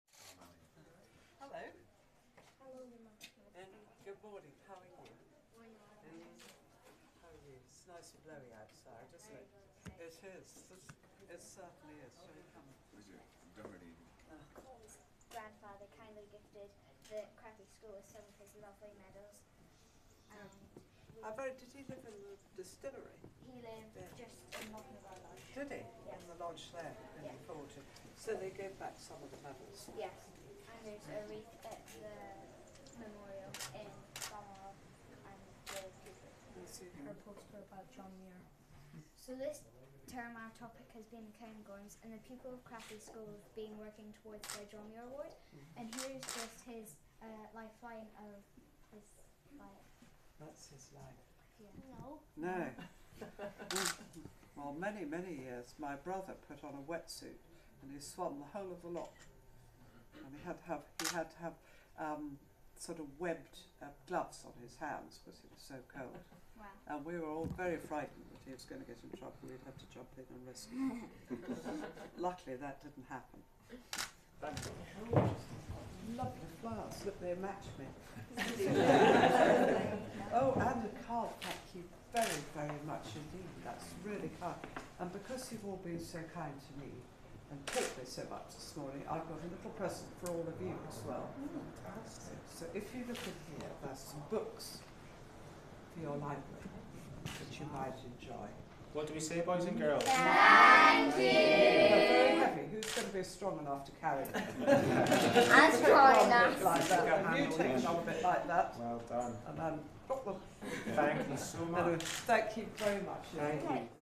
Camilla misses royal wedding to visit school in Scotland
Camilla Duchess of Cornwall has spent today 400 miles from the royal wedding as she carried out royal wedding duties in Scotland. Prince Charles attended the nuptials of Princess Eugenie and Jack Brooksbank this morning with his sons and their families while his wife remained at Balmoral. The Duchess is today watching a display of Highland dancing during a second day of royal engagements in Aberdeenshire.